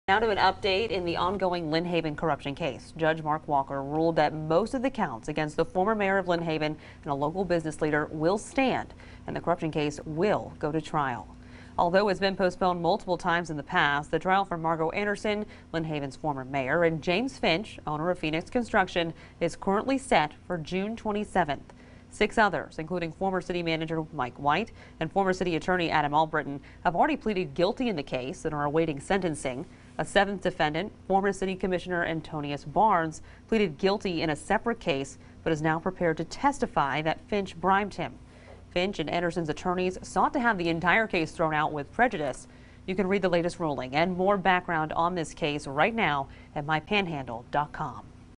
Judge rules Lynn Haven corruption case will go to trial
Judge Mark Walker ruled Tuesday that most of the counts against the former mayor of Lynn Haven and a local business leader will stand and the corruption case will go to trial.